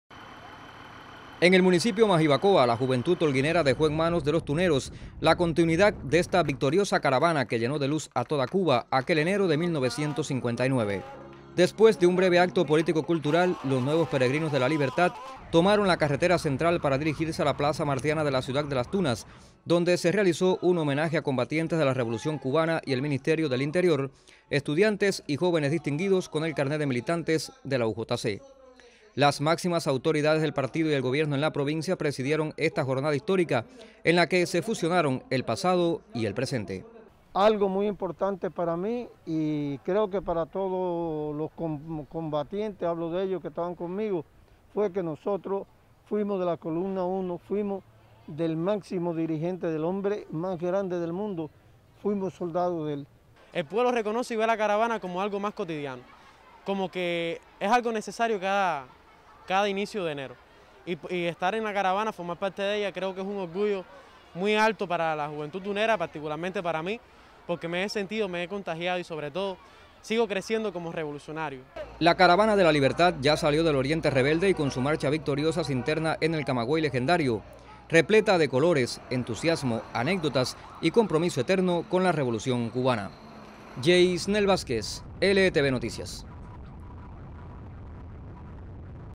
Reeditan en Las Tunas paso de la Caravana de la Libertad
La juventud tunera protagonizó este 4 de enero la reedición del paso de la Caravana de la Libertad por esta oriental provincia. Como hace 63 años el pueblo salió a reafirmar su compromiso con la Revolución y el Socialismo.